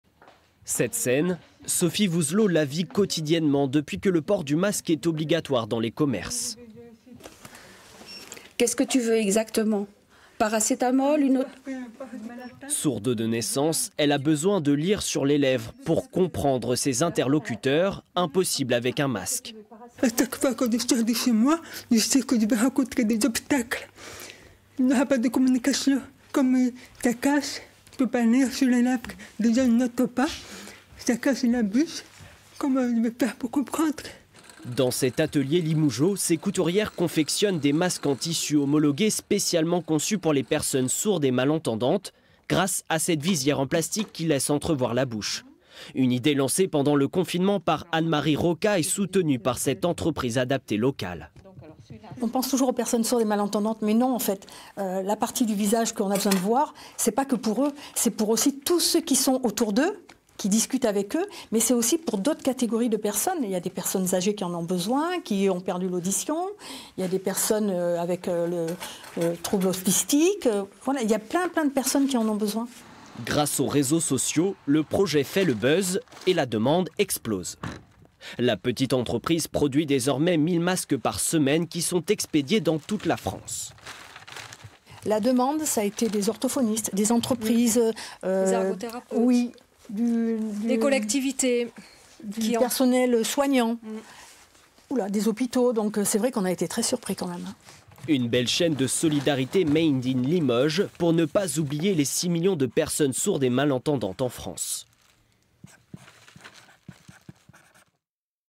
Limoges : des masques pour sourds et malentendants homologués
Il fallait y penser, des masques pour les sourds et malentendants ont été créés par une association de Limoges. Elle mène des actions pour développer la pratique de la langue de signes. Ces masques sont transparents pour permettre de lire sur les lèvres. Les 300 premiers exemplaires fabriqués et homologués ont tous été vendus très rapidement;

Sophie Vouzelaud, marraine de la Fédération Nationale des Sourds  de France
Anne-Marie Roca, présidente de l'association LSF PI TOUS

Reportage : Antoine Jegat et Valérie agut.